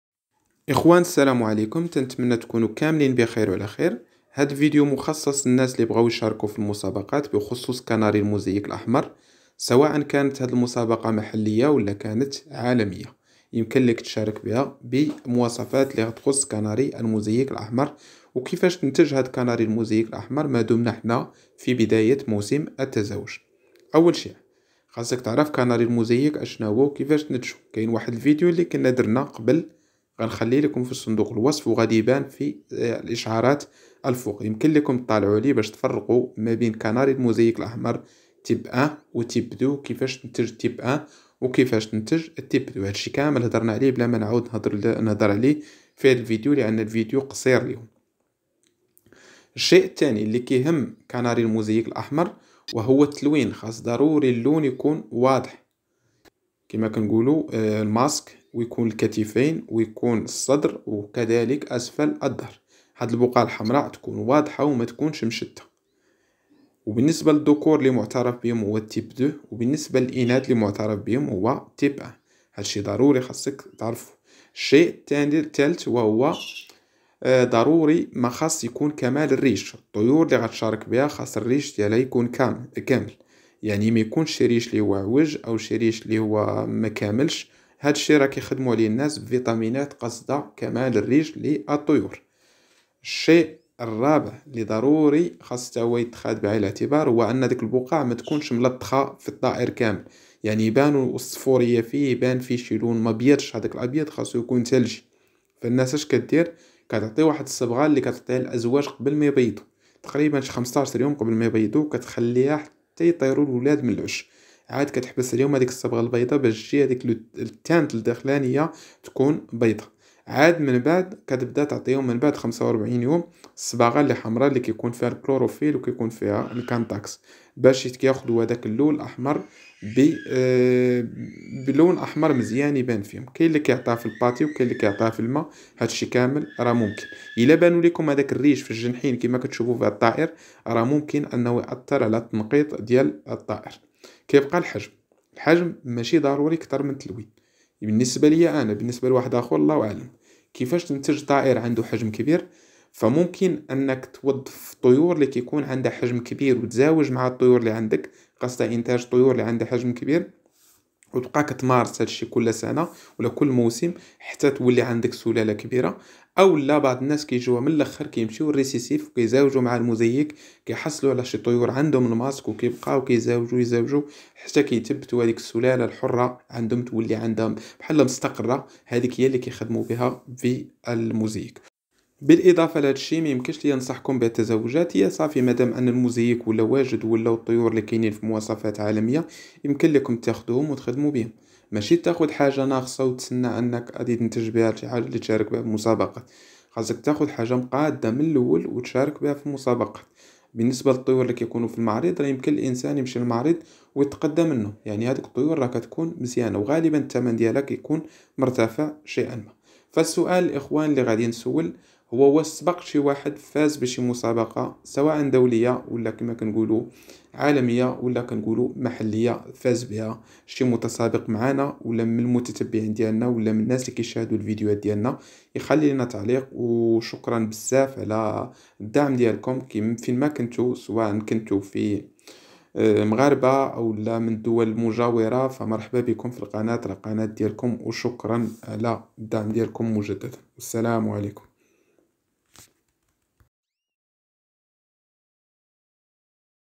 انتاج كناري الموزاييك و خطة الفوز بالمسابقات الدولية والفائز لسنة 2023
كناري الموزايبك الأحمر و خطة الفوز بالمسابقات المحلية و الدولية.